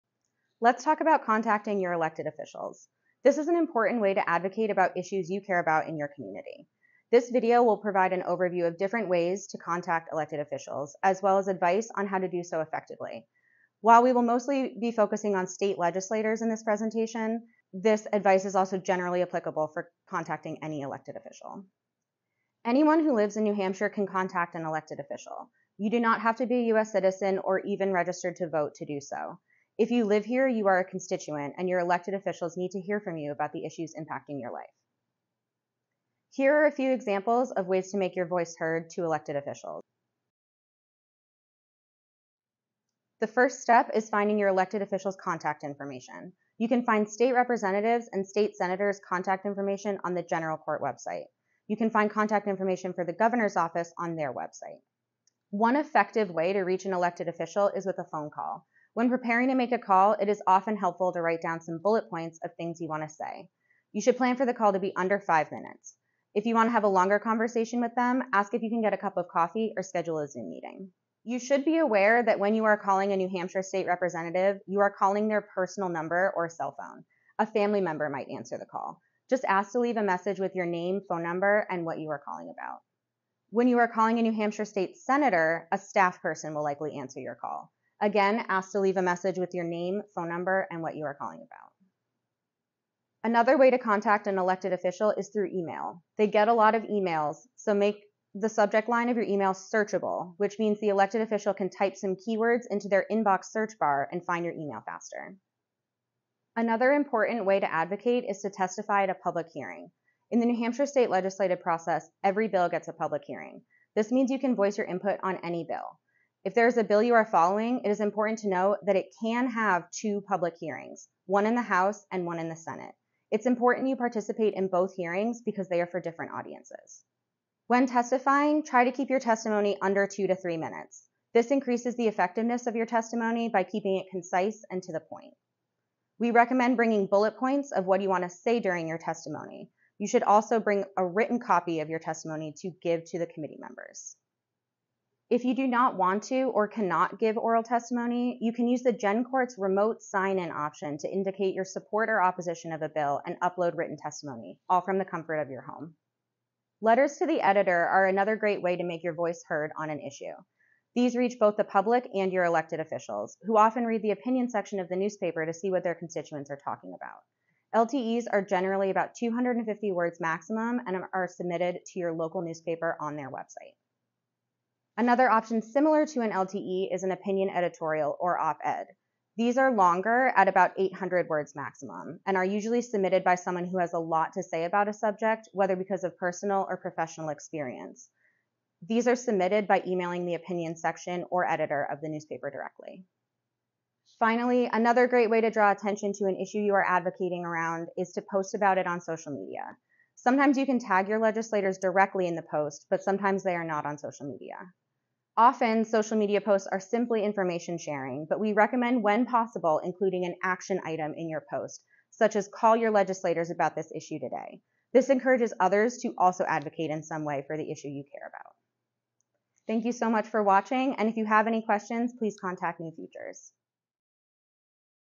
Contacting your Legislators
Contacting your elected officials is an important way to advocate for issues you care about in your community. Learn about the many ways you can contact your elected officials, ranging from phone calls and emails to testifying and writing to newspapers.